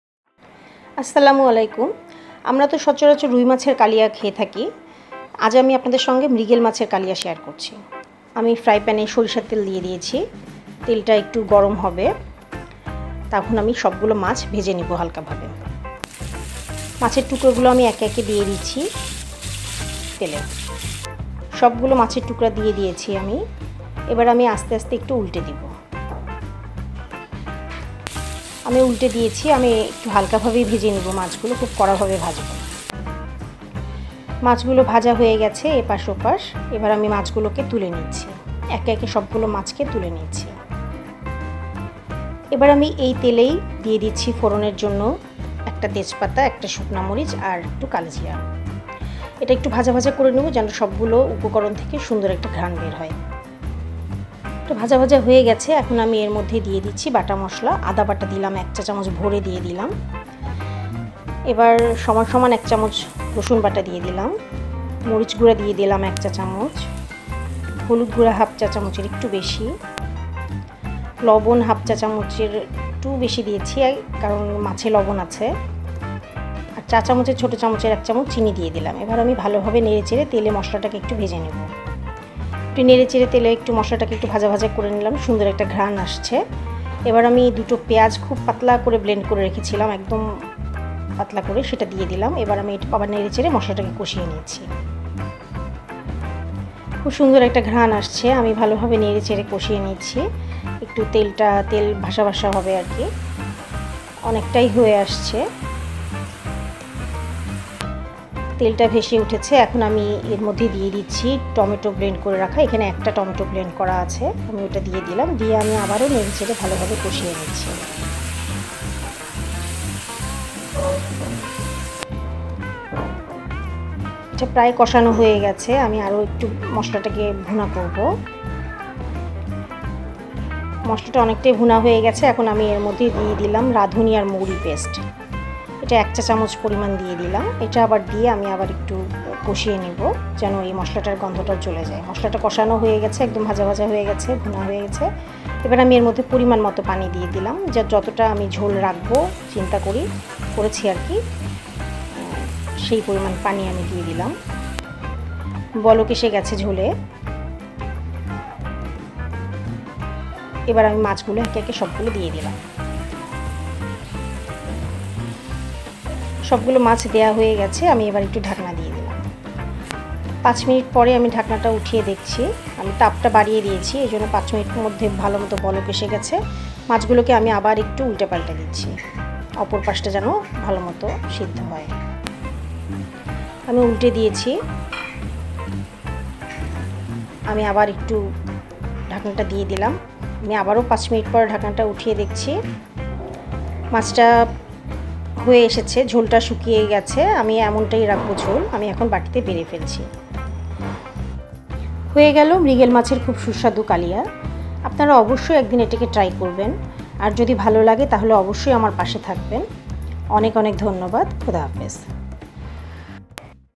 মৃগেল মাছ এভাবে এই প্রথম রান্না করলাম Mrigel Fish Curry
Today's recipe is Mrigel Fish Curry in a different way. This is very unique and testy. Hope you all will like this recipe.